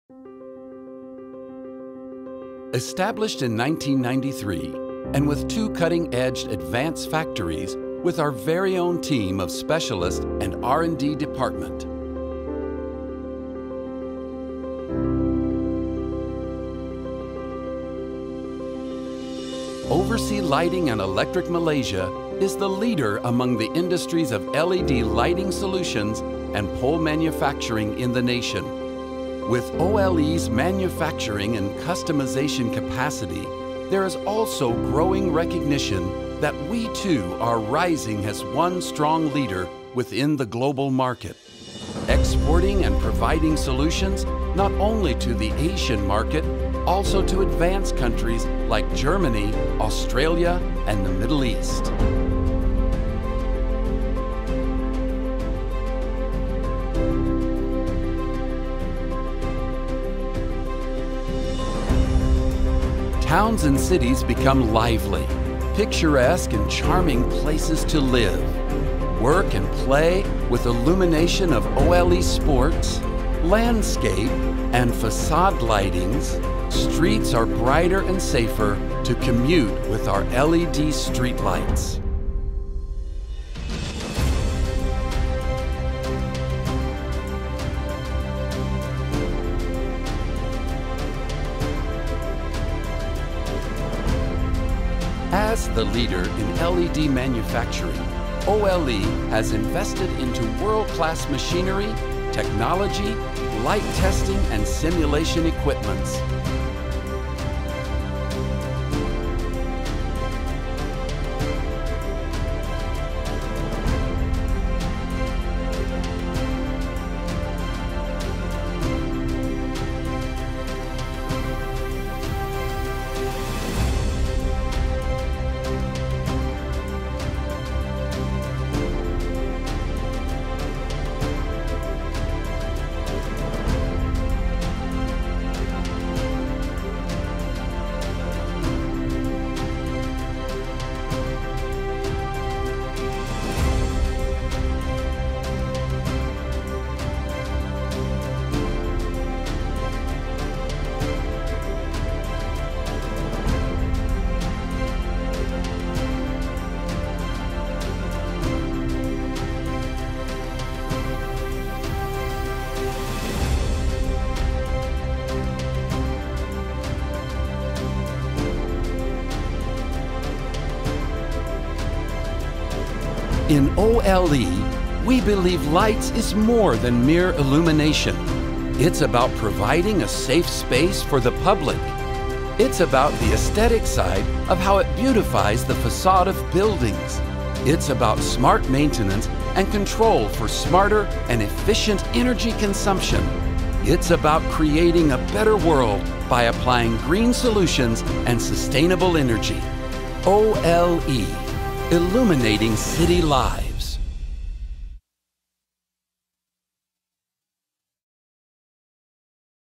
OLE Malaysia’s First LED Lighting & Pole Maker
Oversea Lighting & Electric (M) Sdn Bhd celebrates over two decades of success and exponential growth as Malaysia’s one stop total LED lighting & pole solutions company. Since the company’s inception in 1993 by founder Dato' Wong See Keong DIMP, Oversea Lighting & Electric (M) Sdn Bhd is the current market leader in Malaysia’s LED lighting & pole industry thanks to their team of specialists, technicians and employees. Based in a 165,000 sq feet factory that also houses a dedicated R & D teams, experimentations with the latest technology and innovations in materials, manufacturing as well as customizations allows the company to be a pioneer in a competitive industry.

The Company is also registered with local authorities, SME Corporation Malaysia, Ministry of International Trade and Industry of Malaysia, Ministry of Finance, Intellectual Property Corporation of Malaysia, Sirim QAS International Sdn Bhd, Ikram QA Services Sdn Bhd, JKR Electrical Material Approved List, Malaysian Brand Certification Scheme and Branding Association of Malaysia. OLE has ventured into the business of IoT following the government’s vision of Industry4WRD in alignment with Industry 4.0. We have designed and developed Smart Lighting Systems and more. Our cloud based Smart Systems provide a complete solution to monitor and manage your assets with IoT infrastructure. Our products are approved by SIRIM and MCMC.